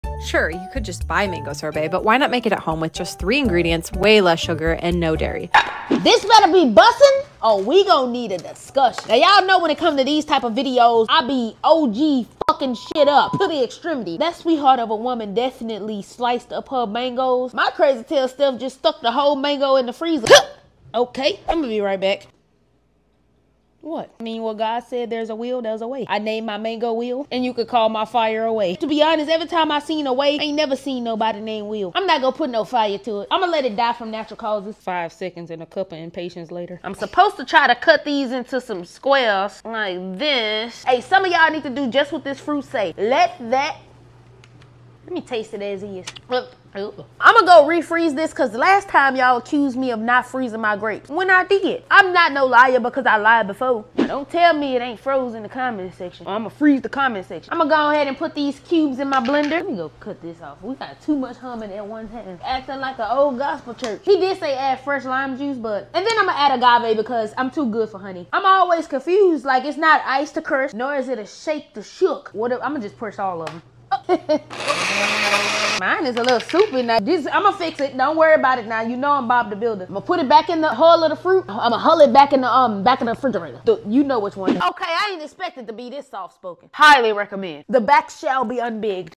MANGO LIME SORBET 🥭 🍨
MANGO LIME SORBET 🥭 🍨 teecee tries it too Tuesday is in full effect! Y’all know I wanted a piece as soon as I seen it.🥹!